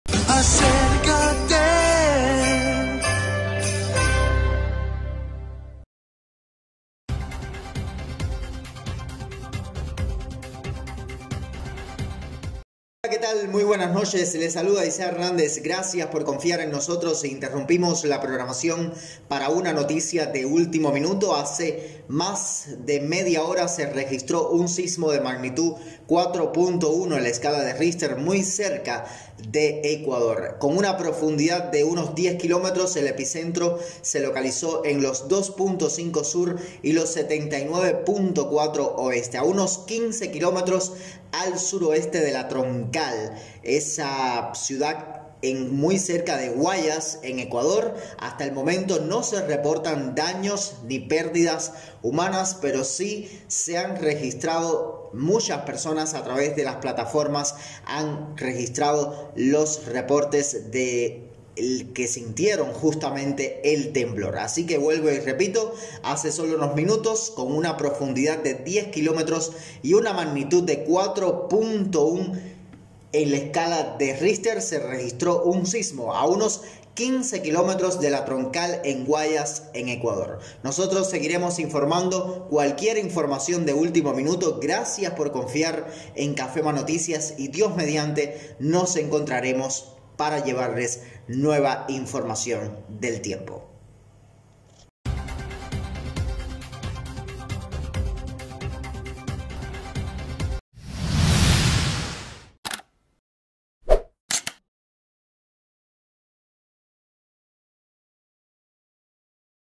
ALERTA DEL TIEMPO "BOLETIN 10PM SOBRE UNA ALERTA DE SISMICA" [VIERNES 4/12/20]
EN ESTOS MOMENTOS EMITIMOS UNA ALERTA SISMICA SOBRE UN TERREMOTO LEVE DE MAGNITUD 4.1 - Provincia del Azuay, 15 km al SO de La Troncal (Cañar), Ecuador.

SIGUENOS TODOS LOS DIAS PARA MAS INFO 👉@cafematvnoticias